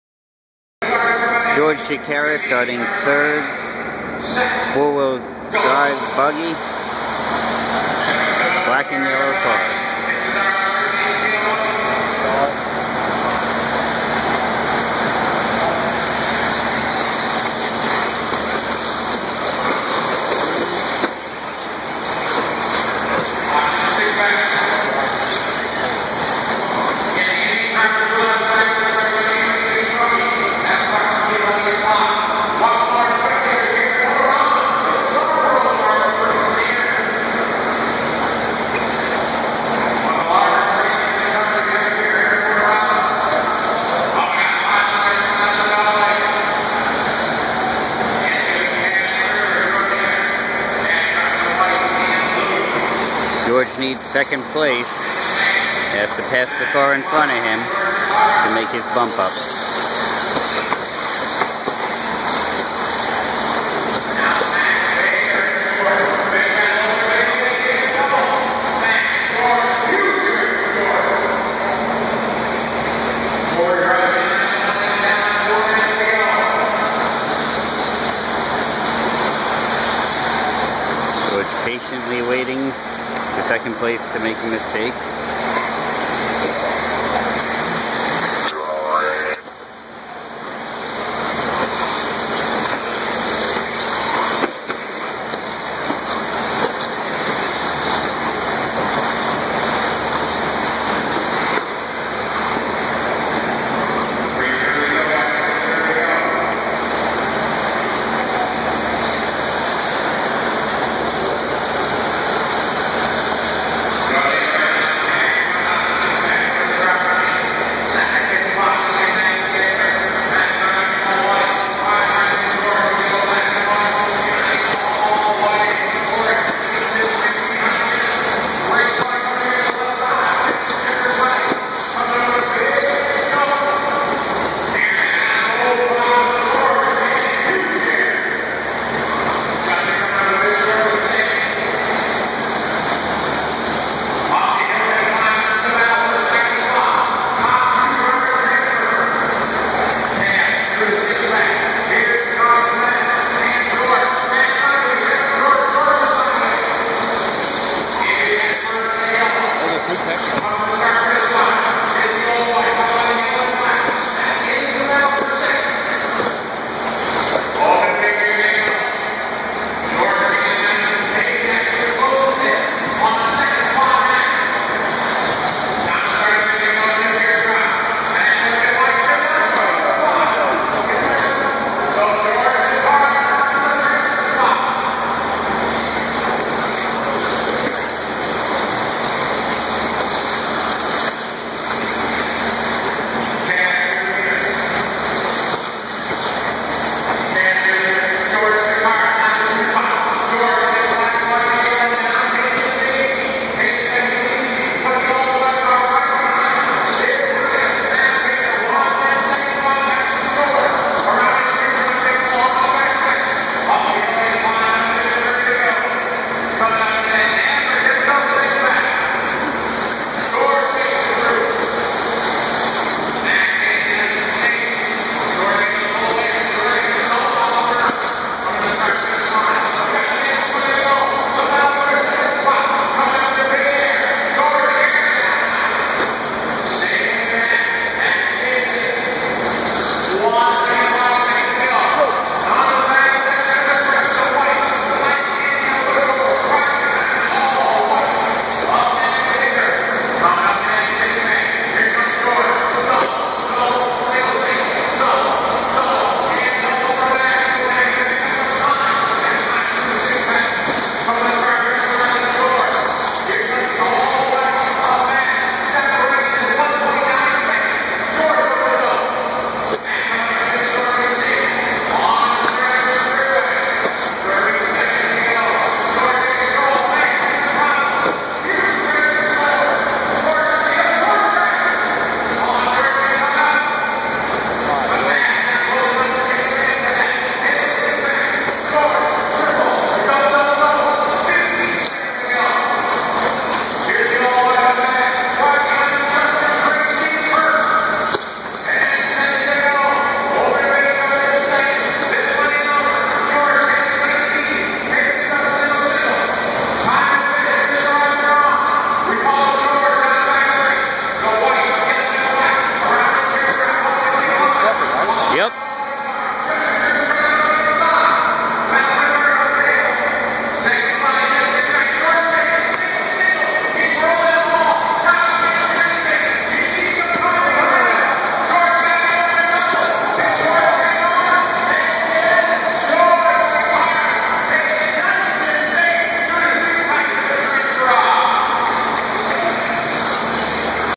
Motorama 2013 Buggy Expert Main
Team Hobbymasters / Venom Racing driver George Tsakiris showing incredible patience and driving strategy as he works to maintain a second place bump-up position at Motorama.